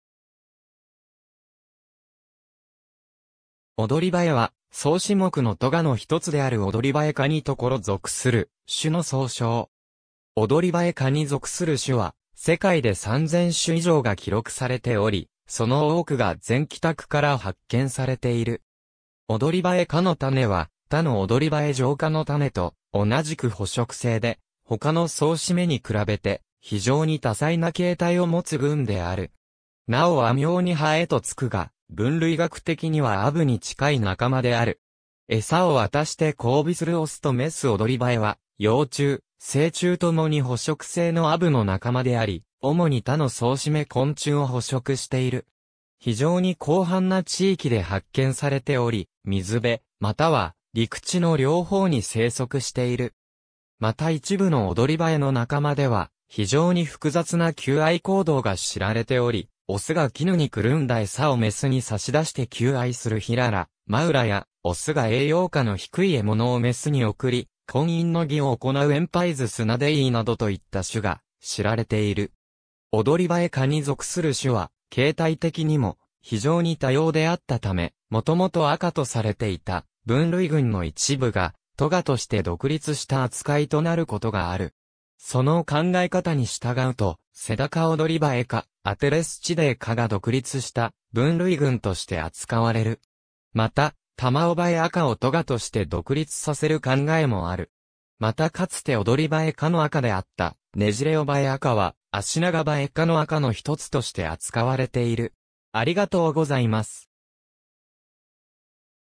オドリバエ
オドリバエは、双翅目の科の一つであるオドリバエ科（Empididae）に所属する種の総称。
オドリバエ科に属する種は、世界で3000種以上が記録されており、その多くが全北区から発見されている。
オドリバエ科の種は、他のオドリバエ上科の種と同じく捕食性で、他の双翅目に比べて非常に多彩な形態をもつ群である。
なお和名にハエとつくが、分類学的にはアブに近い仲間である。
餌を渡して交尾するオスとメス オドリバエは、幼虫、成虫共に捕食性のアブの仲間であり、主に他の双翅目昆虫を捕食している。
非常に広範な地域で発見されており、水辺または陸地の両方に生息している。
また一部のオドリバエの仲間では、非常に複雑な求愛行動が知られており、オスが絹にくるんだ餌をメスに差し出して求愛する Hilara mauraや、オスが栄養価の低い獲物をメスに送り、「婚姻の儀」を行う Empis snoddyiなどといった種が知られている。
オドリバエ科に属する種は形態的にも非常に多様であったため、もともと亜科とされていた分類群の一部が、科として独立した扱いとなることがある。
その考え方に従うと、セダカオドリバエ科（Hybotidae）、Atelestidae科が独立した分類群として扱われる。
またタマオバエ亜科（Brachystomatinae）を科として独立させる考えもある。
またかつてオドリバエ科の亜科であったネジレオバエ亜科（Microphorinae）は、アシナガバエ科の亜科の一つとして扱われている。